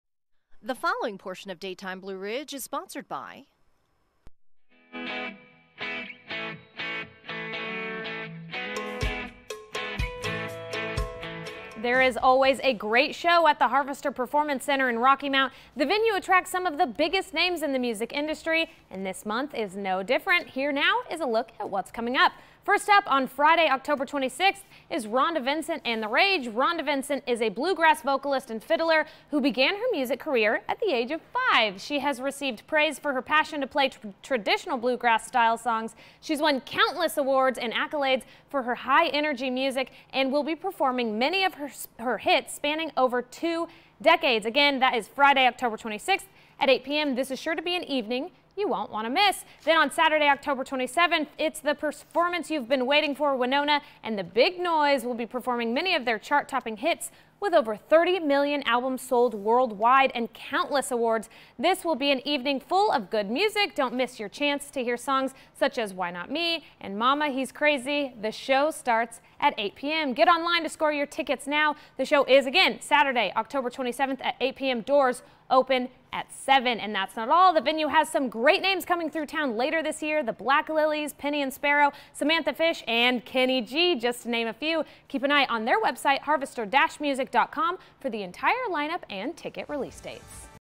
Some Great Acts Coming to the Harvester Performance Center
A Preview of Some Great live music coming to the Harvester Performance Center in Rocky Mount in the next few weeks, and how you can get tickets!